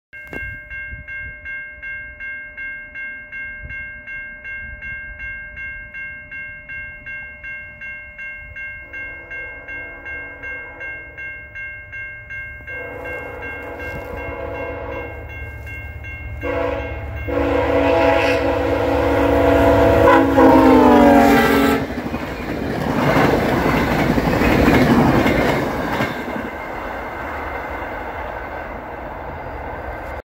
(Emergency Horn/Nice Driver) (Hep-2 Buffer 4118!!) VIA 73 - 913 With Budd Cars In Lynden Ontario
Rear Buffer Drummond Manor had been swapped for Hep-2 Budd Coach 4118!